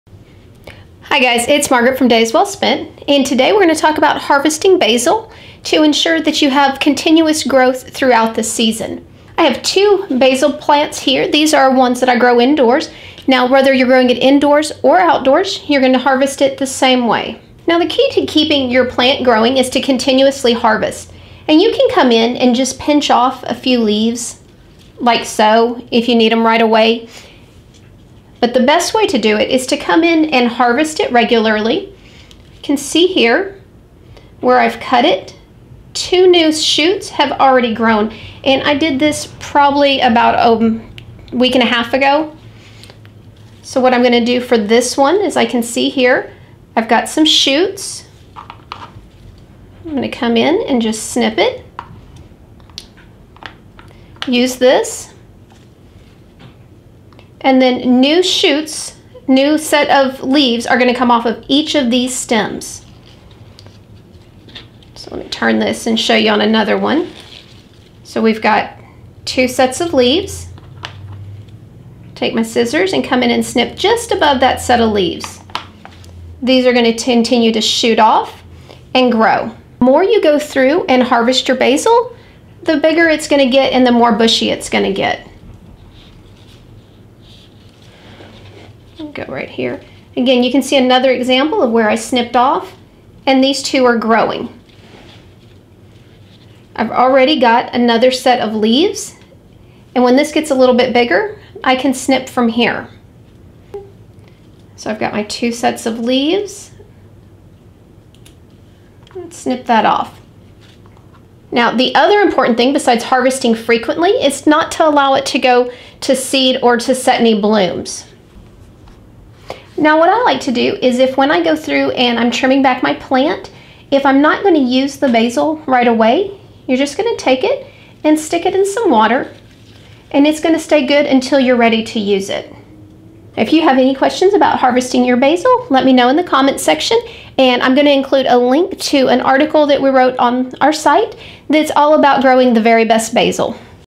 How to Harvest Basil for Continuous Plant Growth
Basil, How to Harvest Basil and Ensure Continuous Plant Growth. In this video, I'll show you how to harvest basil and ensure your plant grows better than ever.